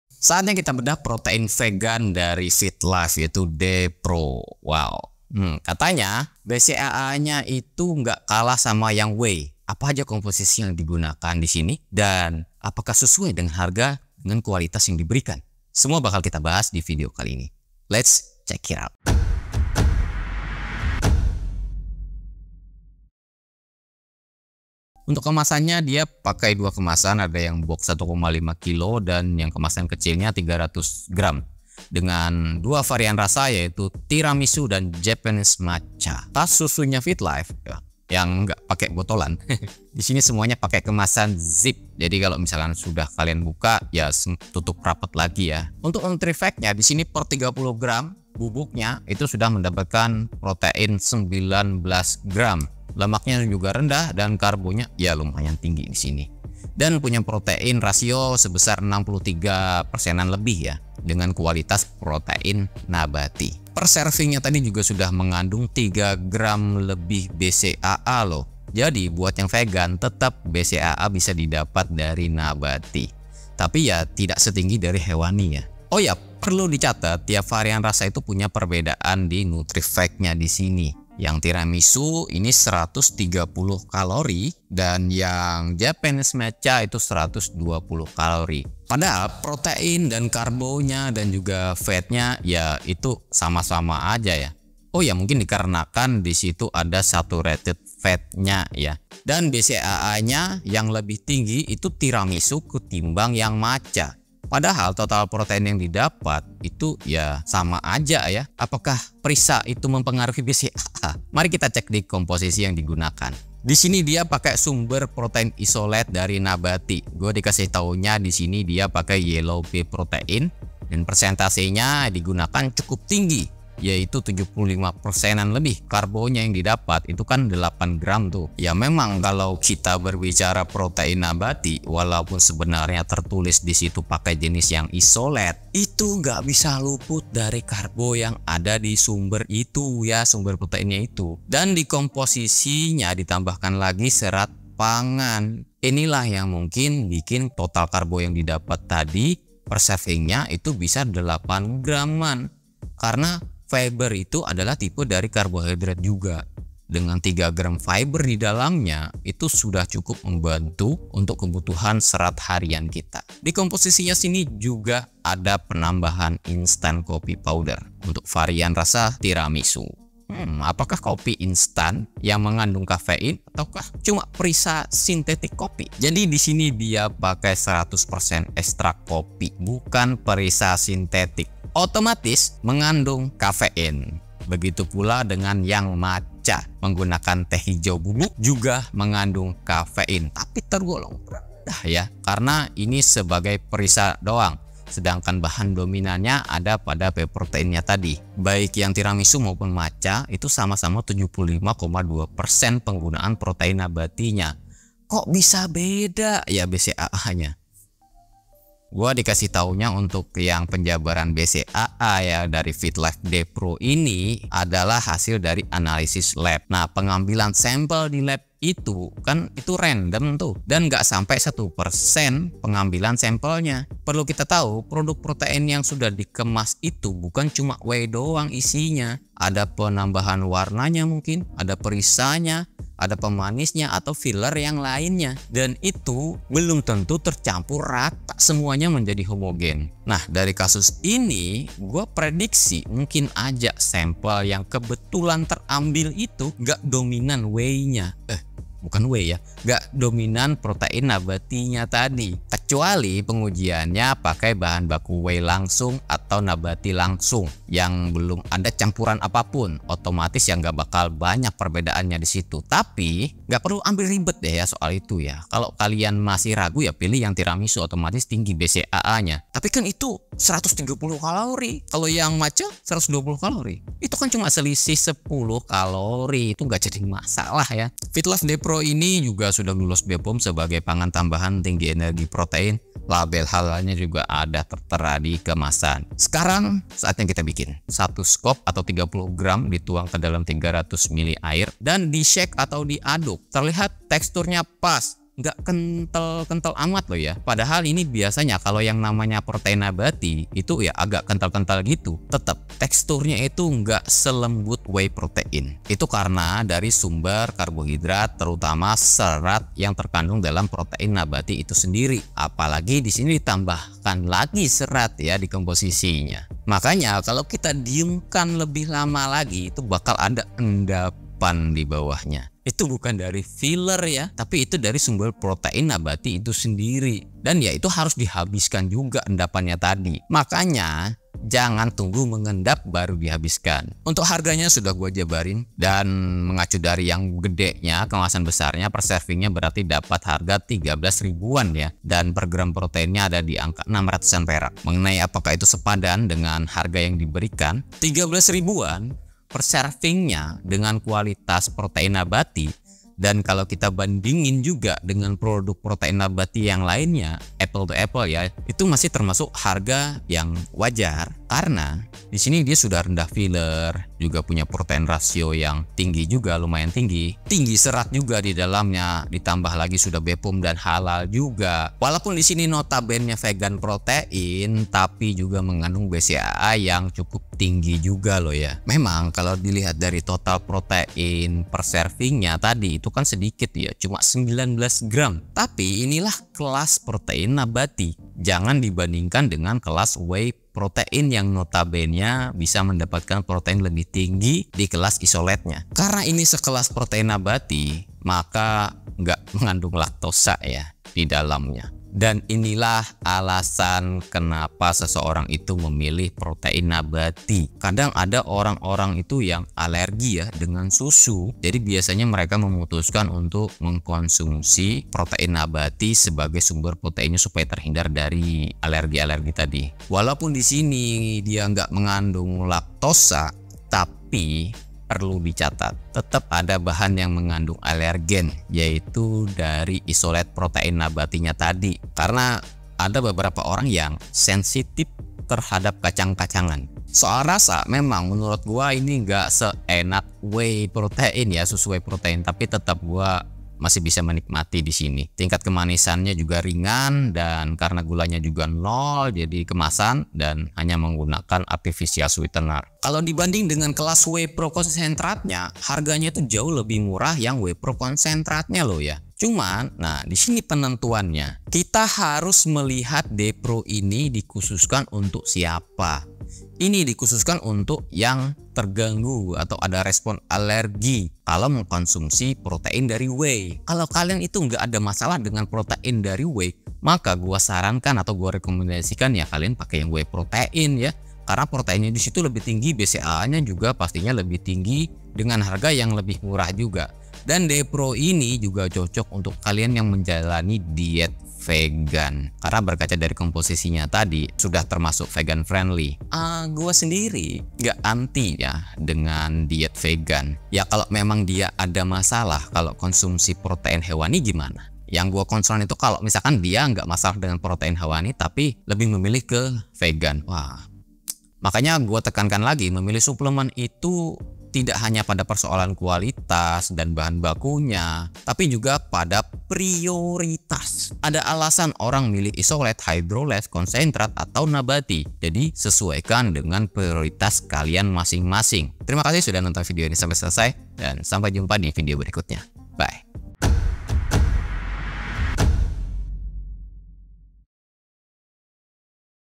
ISOLATE KOK KARBONYA MASIH TINGGI..?! FITLIFE DPRO PROTEIN NABATI
Saatnya kita bedah protein vegan, fitlife DPRO. Hmm katanya BCAA nya gak kalah sama yang whey, apa aja komposisi yang digunakan disini, dan apakah sesuai harga dengan kualitas yang diberikan? Semua akan kita bedah di video kali ini, lets check it out.

SUBSRCIBE channel ini...